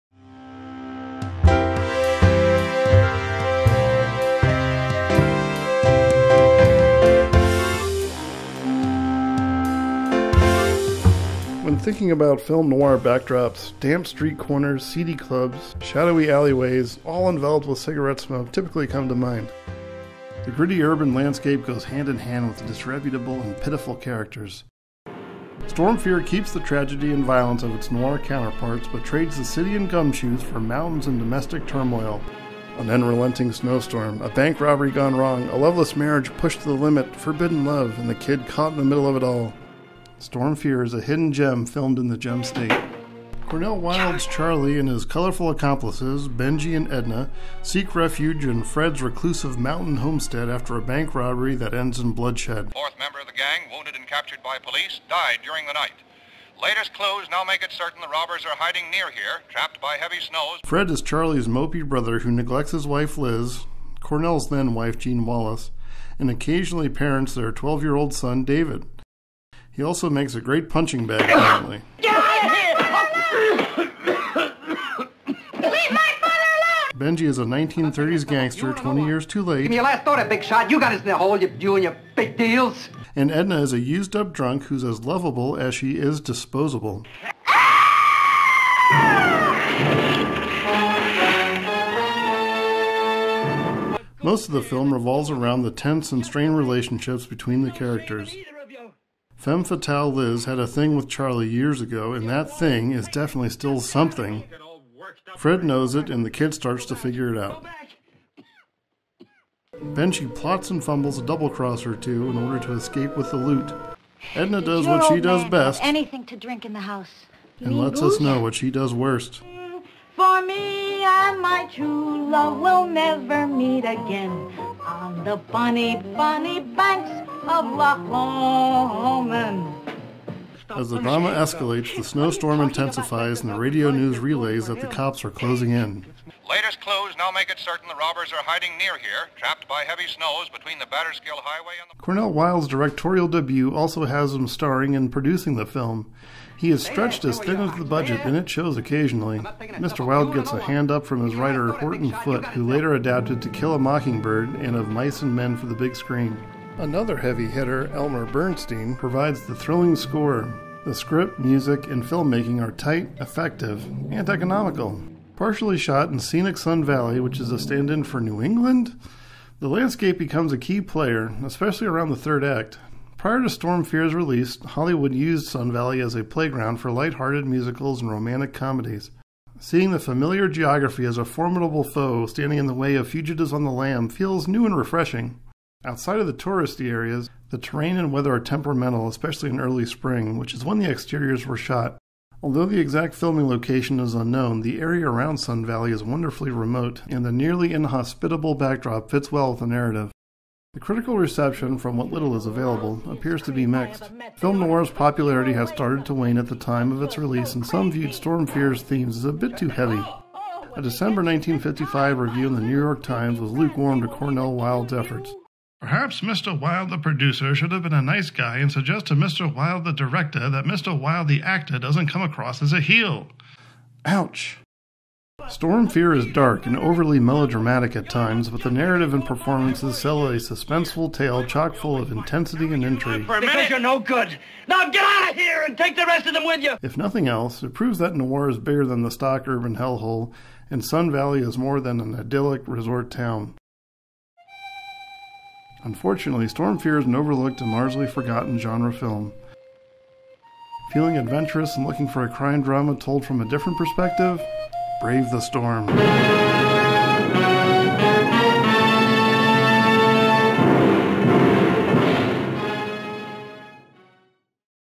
4. Storm Fear
Film Noir takes Idaho by Storm in this forgotten crime drama set in Sun Valley. Not sure where exactly it was filmed, so I wandered around the SRA scenery for a bit.